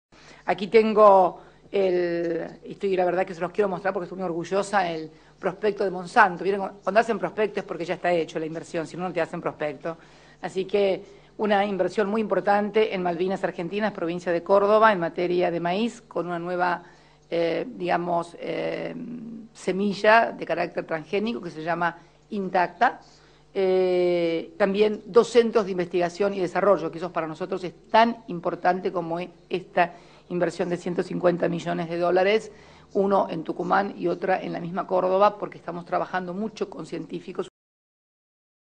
CRISTINA FERNÁNDEZ - Consejo de las Américas - 2012
Sobre MONSANTO en NUEVA YORK.